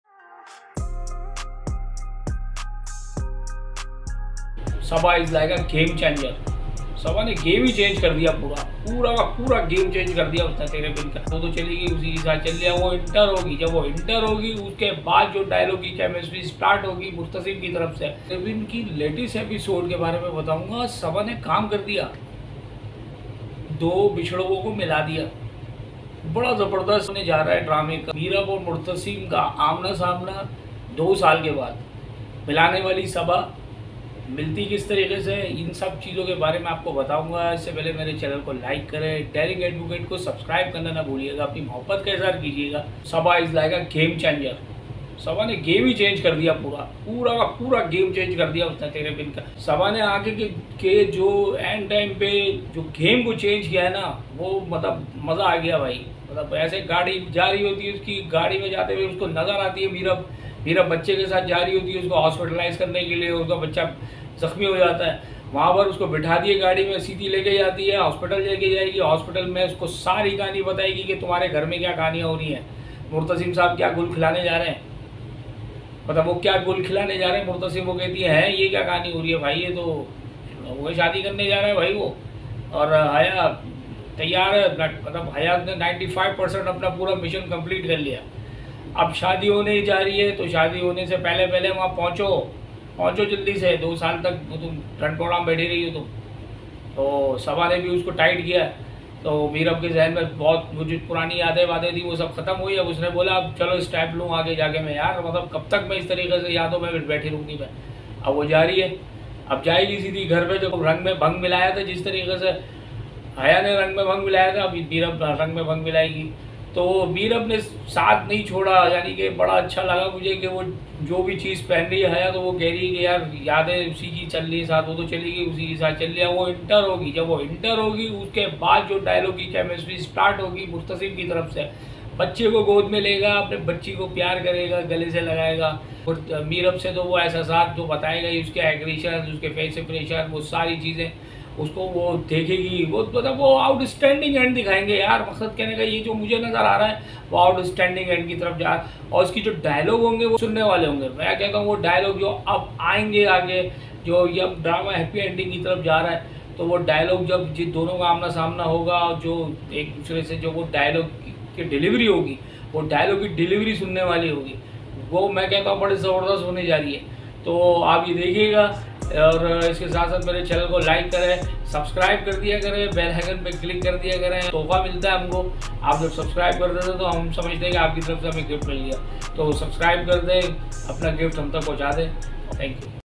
Tere Bin Ep 57 - Teaser promo review - Yumna Zaidi - Wahaj Ali
Hi, Friends I am daring Advocate, i am get the analysis predict and review  from tere bin episode 57 episode.

Drama Info

Writer: Nooran Makhdoom
Director: Siraj ul Haq
Producers: Abdullah Kadwani & Asad Qureshi
Production House: 7th Sky Entertainment

Cast :
Haris Waheed
Agha Mustafa

My channel Publish Videos Focus to, Innovative, Technology, Sports, Movies and Entertainment.